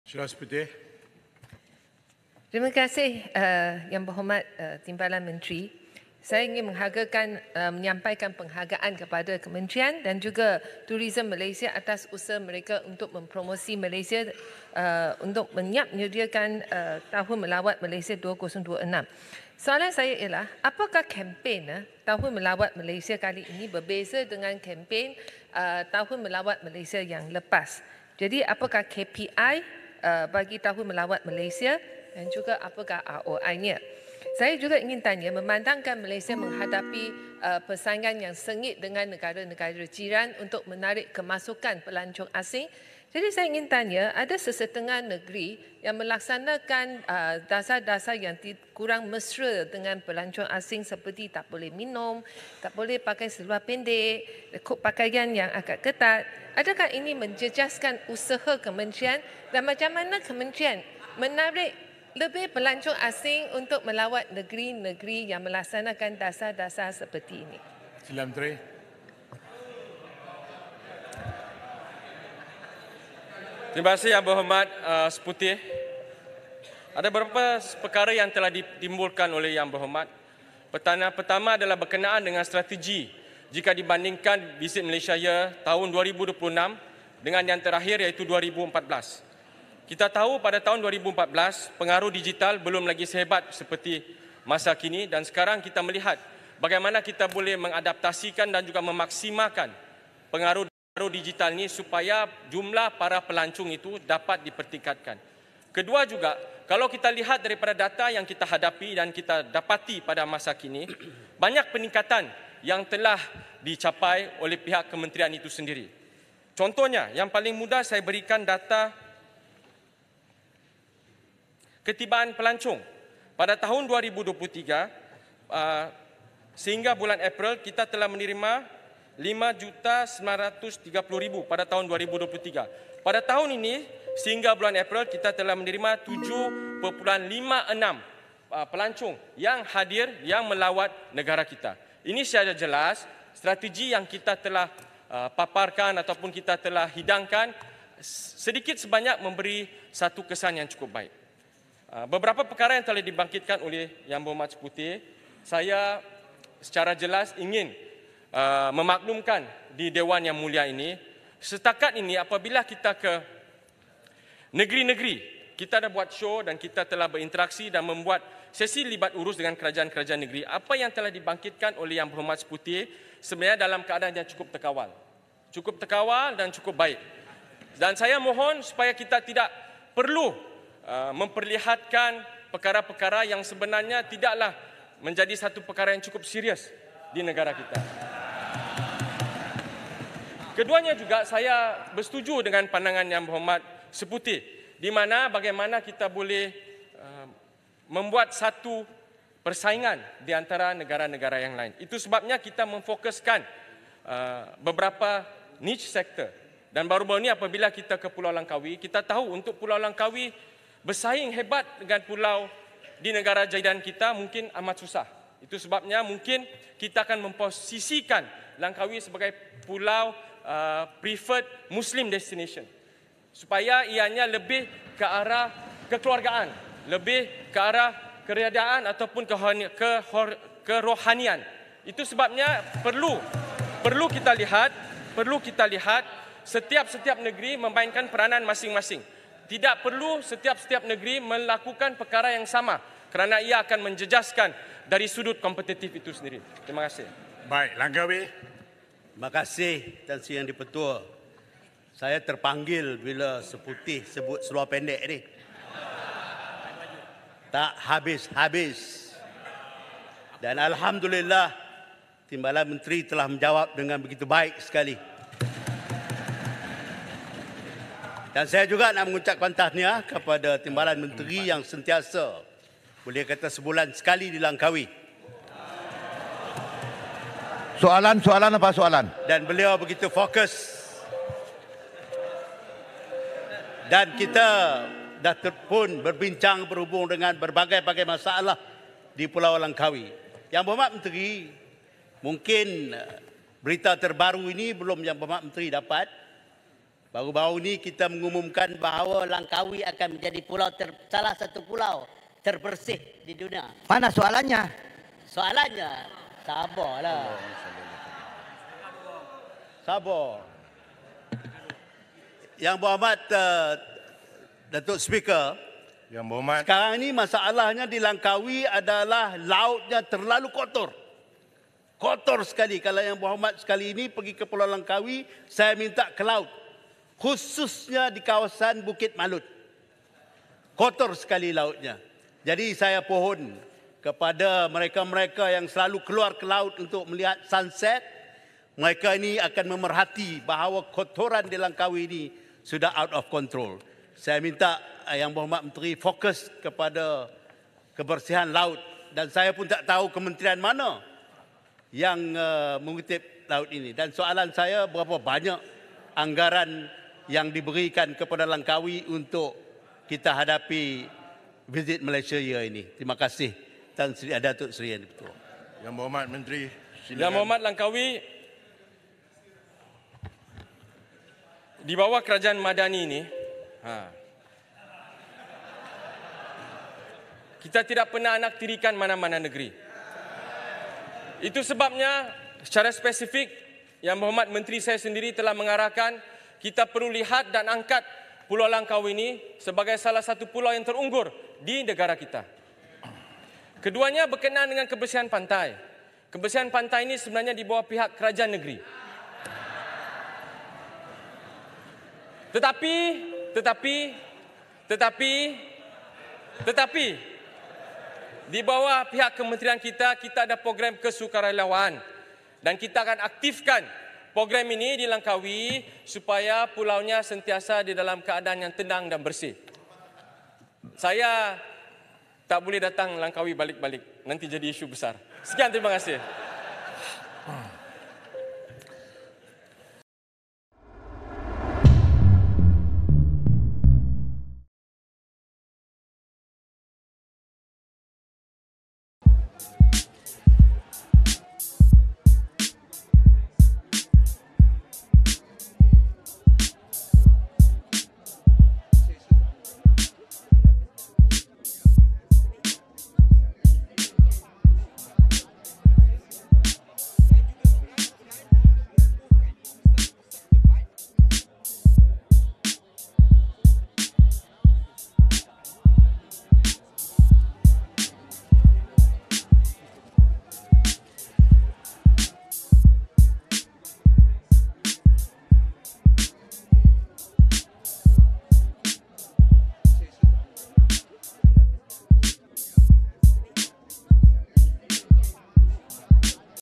'Tak habis-habis Teresa bangkit isu seluar pendek' - MP Langkawi
Ahli Parlimen Seputeh Teresa Kok disindir oleh ahli parlimen Langkawi Mohd Suhaimi Abdullah apabila membangkit isu pelancong berseluar pendek di tempat awam.

Suhaimi juga turut memuji Timbalan Menteri Pelancongan, Seni Dan Budaya Malaysia Khairul Firdaus Akbar Khan menjawab pertanyaan Teresa yang bertanya apa langkah kementerian itu untuk menarik pelancong ke negeri yang mempunyai garis panduan pelancong.